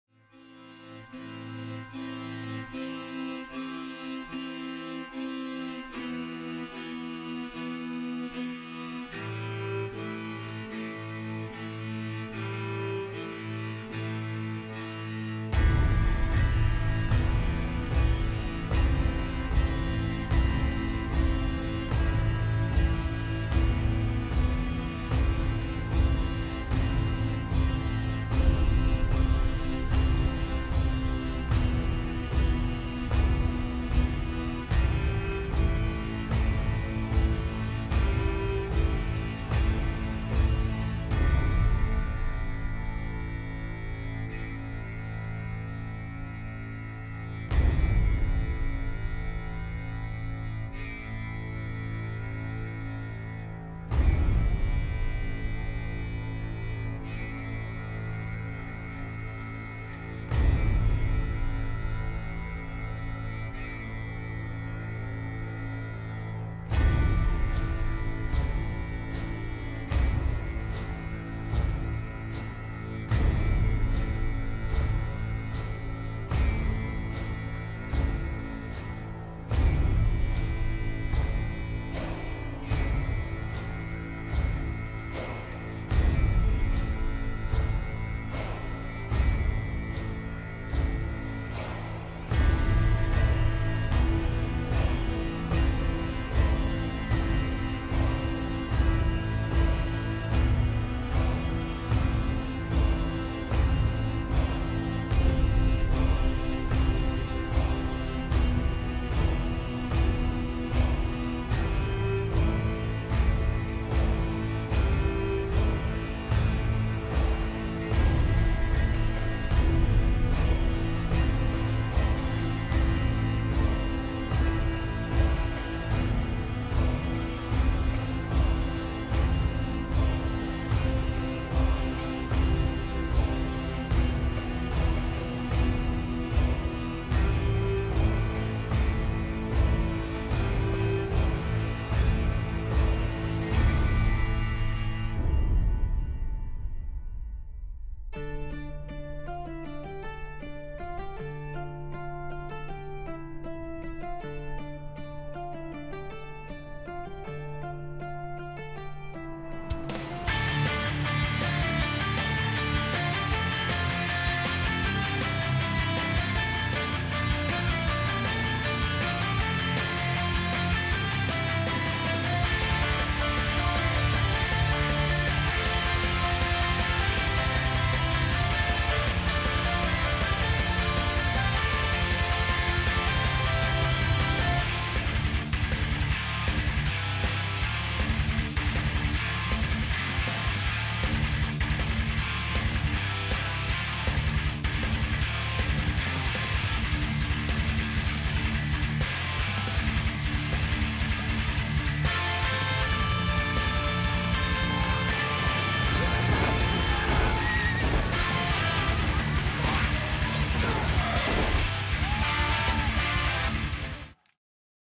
#9 Battle of the bastards, Acrylic on canvas
Vikians rendition of Battle of the bastards from Game of thrones. The epic TV series is coming to an end and Vikian paint's a tribute to the show.

Vindsvept, fantasy music, check him out on
- FAQ, tutorials and ways to get in touch